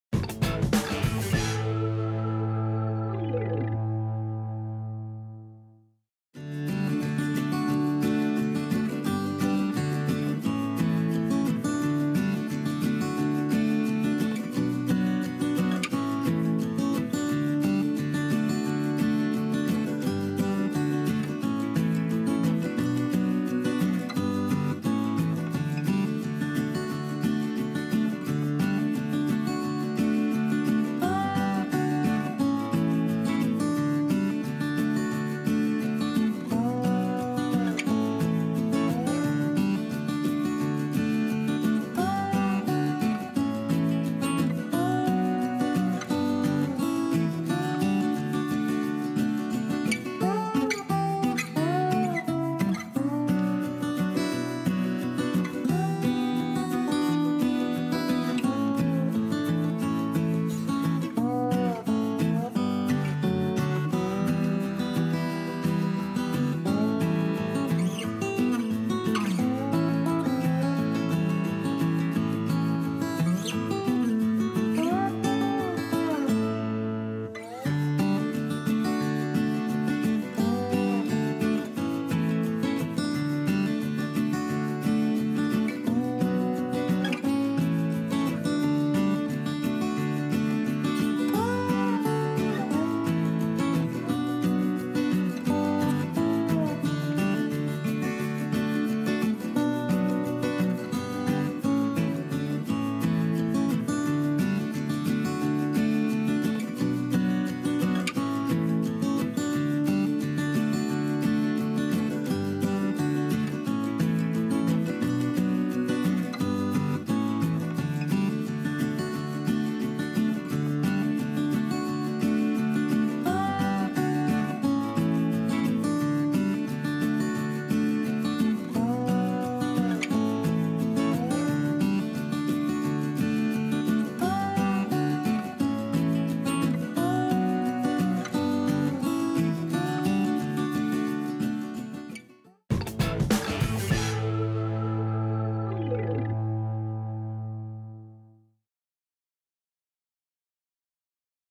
Como Hacer Conejo de Papel- HomeArtTv por Juan Gonzalo Angel
¿Quieres aprender a hacer este lindo conejo de papel? Puede ser un elemento decorativo muy original para Pascua. Si quieres divertirte junto a tus hijos, enséñales a hacer esta manualidad, paso a paso, con este sencillo tutorial. ¿Preparados?